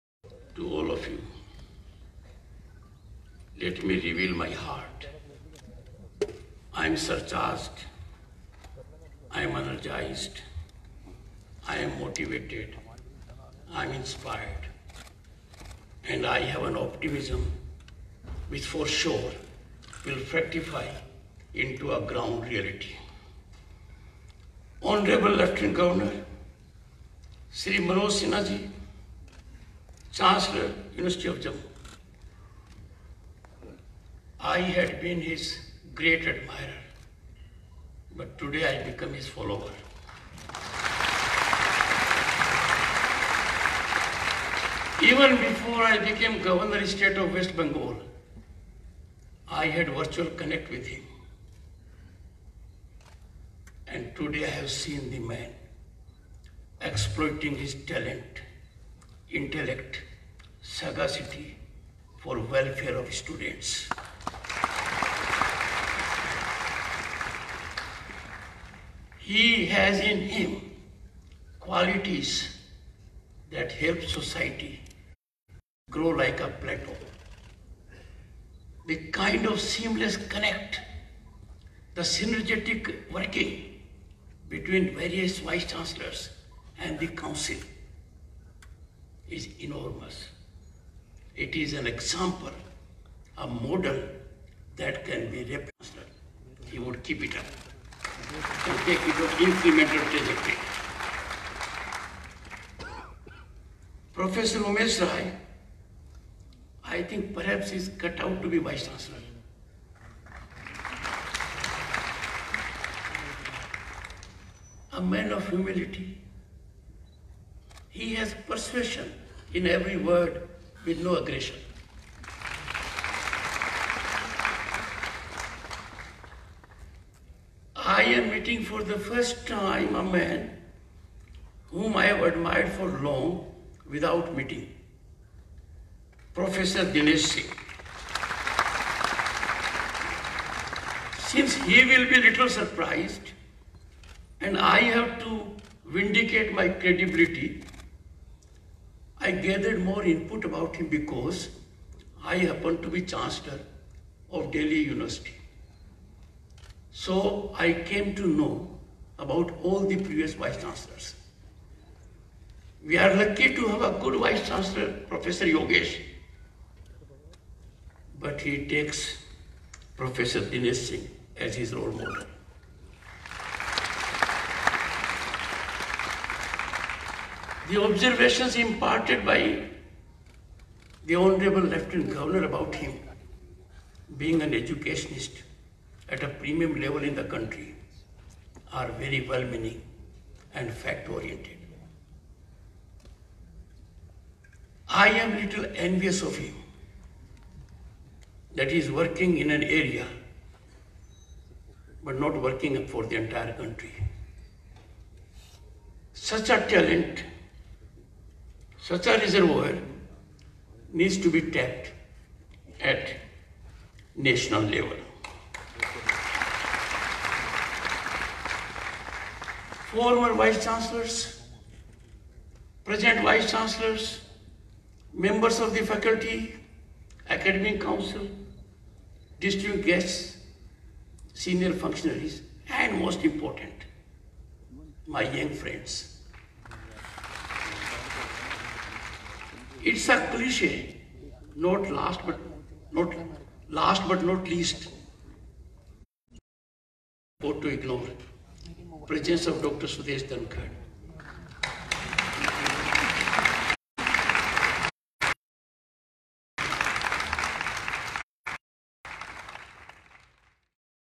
Jagdeep Dhankar on 370 عارضی دفعہ 370 بھی 70 برس تک رہا، جگدیپ دھنکر
نائب صدر جگدیپ دھنکر نے جموں یونیورسٹی میں دفعہ 370سمیت مختلف موضوعات پر گفتگو کرتے ہوئے کہا: ’’جموں و کشمیر میں اصل تعمیر و ترقی دفعہ 370کی منسوخی کے بعد ہی دیکھنے کو ملی۔‘‘